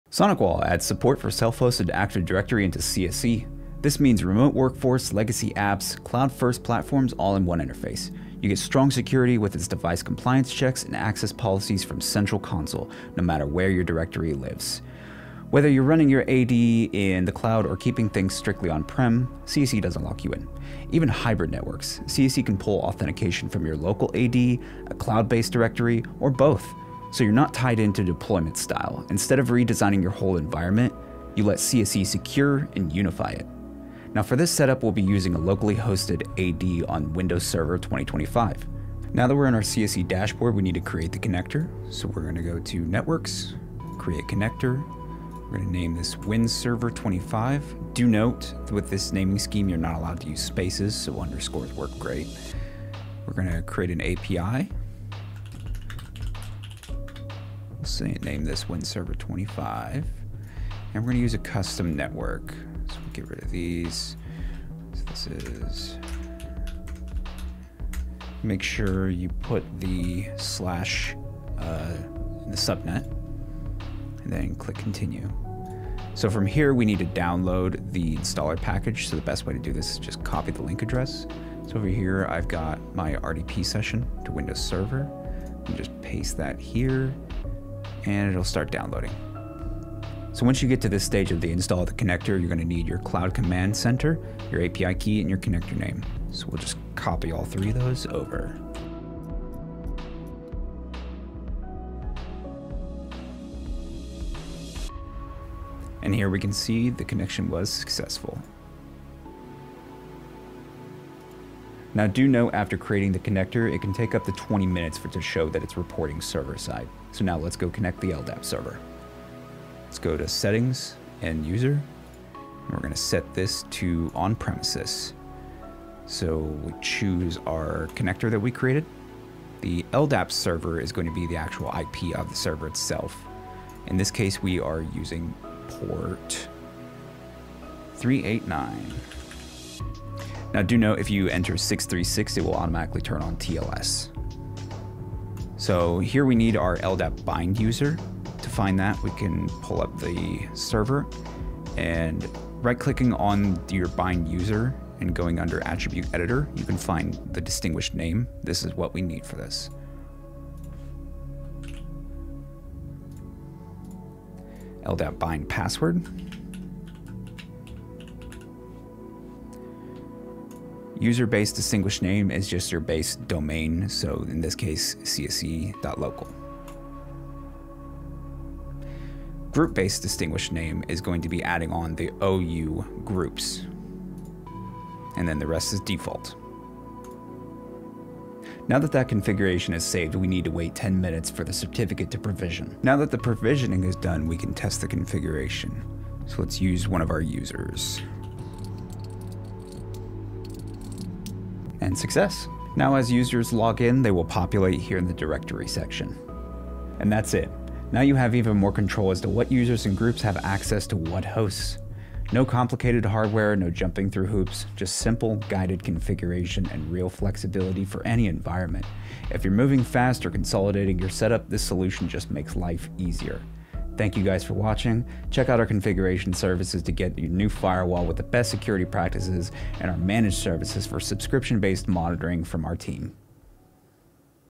SonicWall CSE LDAP Setup | Active Directory Integration Walkthrough
Learn how to set up LDAP integration with SonicWall Cloud Secure Edge (CSE) using a self-hosted Active Directory. In this step-by-step walkthrough, Network Engineer William shows you how to connect your on-prem or hybrid AD environment to CSE.

# Time Stamps #
0:38 - Setting up connector
1:56 - Connecting the LDAP
3:12 - Testing connection

Questions? Talk to a certified SonicWall engineer.